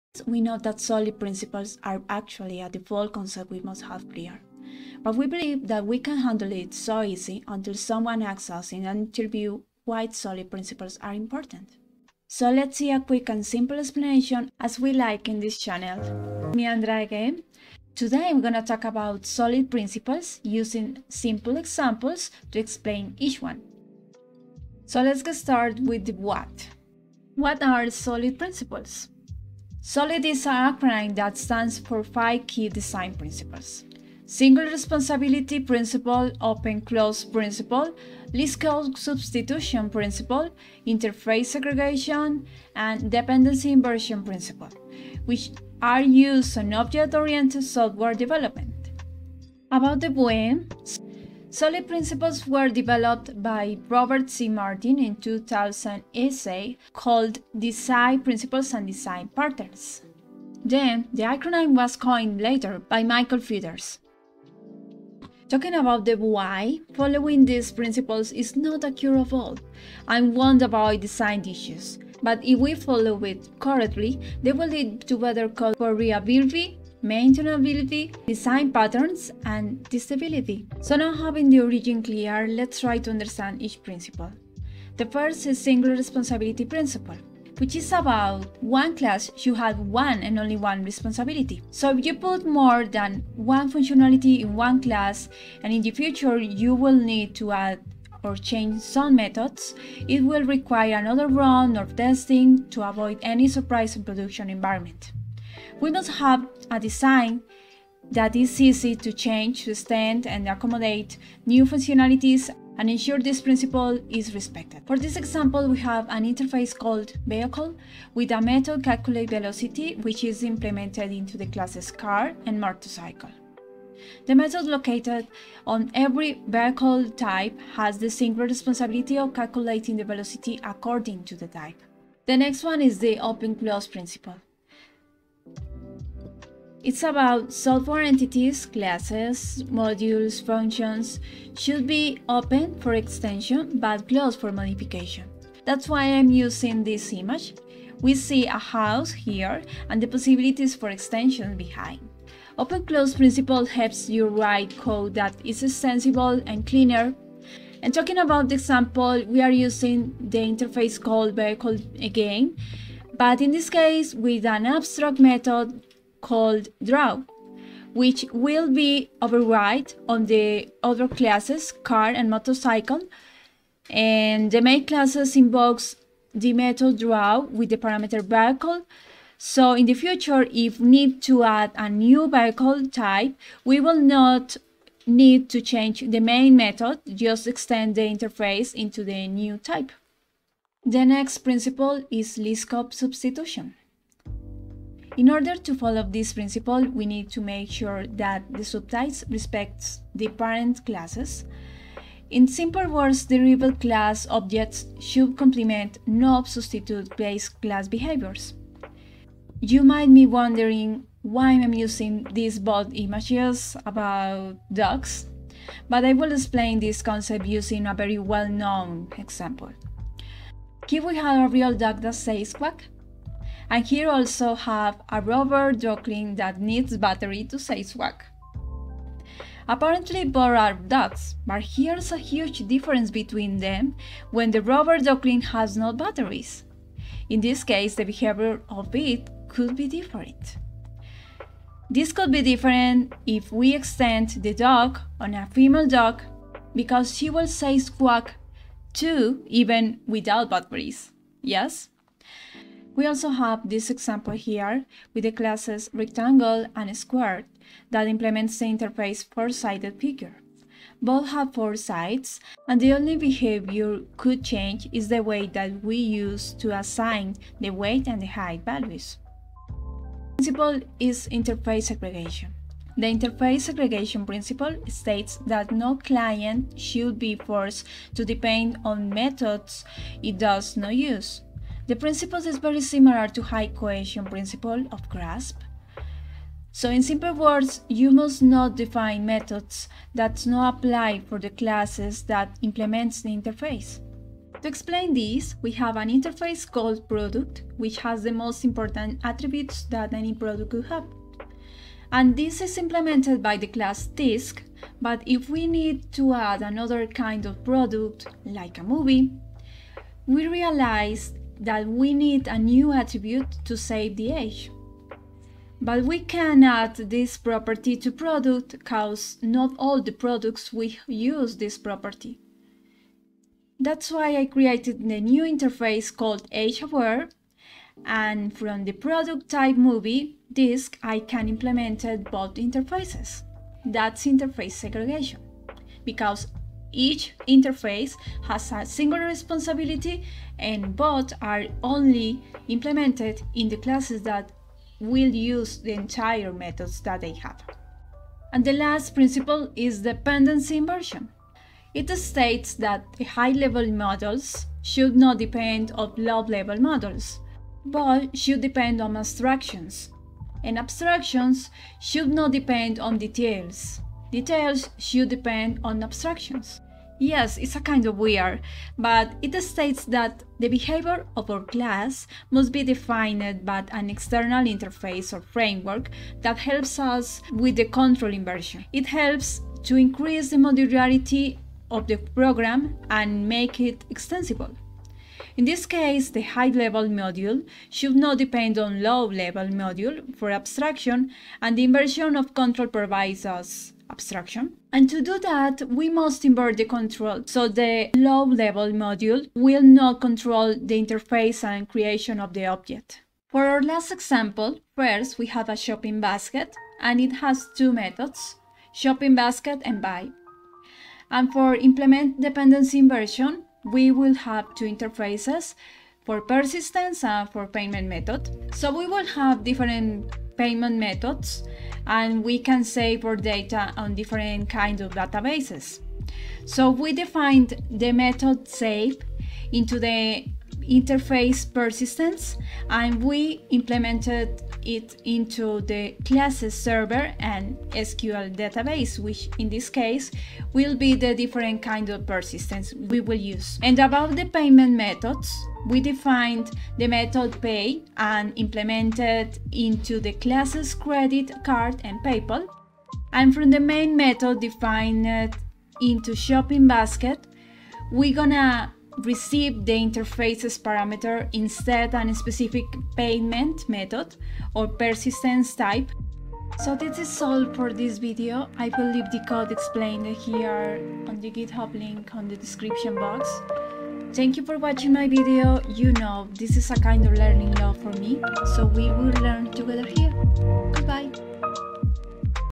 How to implement SOLID principles in java
Let´s learn SOLID design principles with simple examples on JAVA